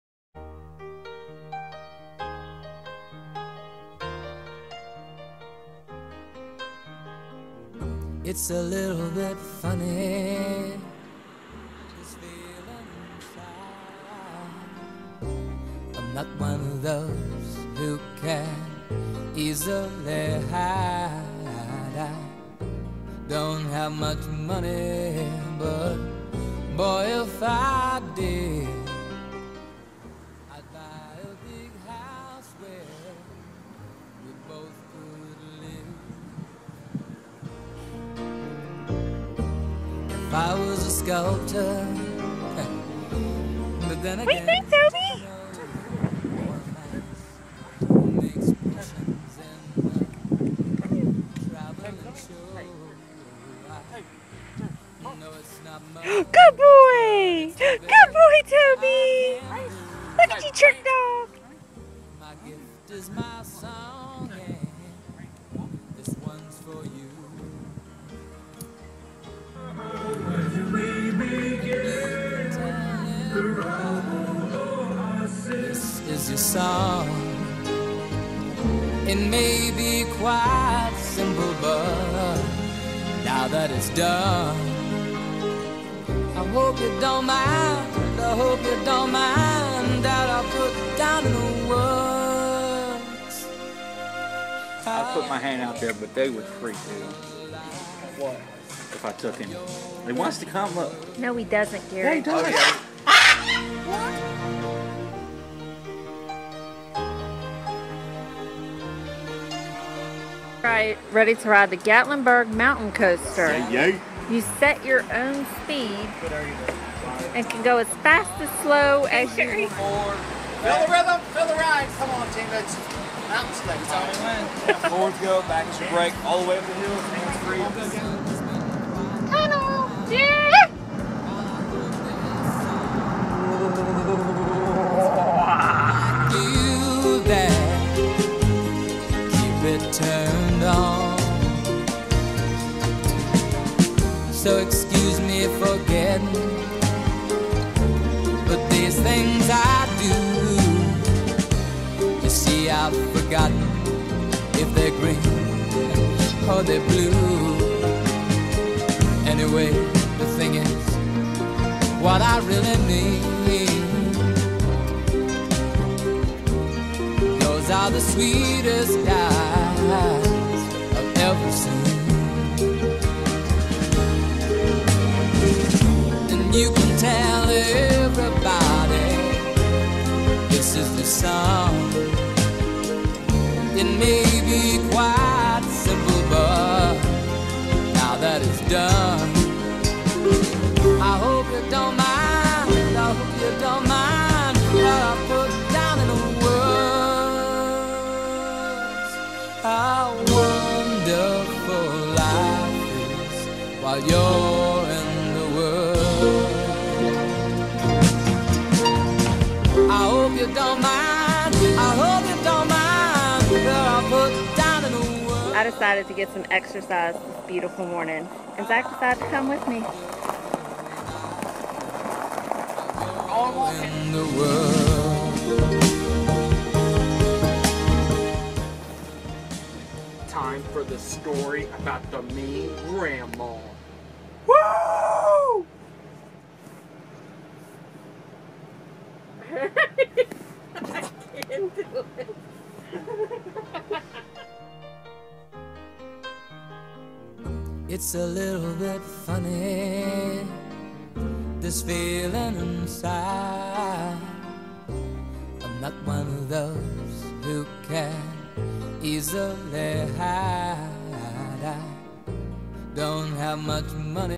Labor Day 2016- TN
Camping at Anchor Down RV Resort with Mom & Dad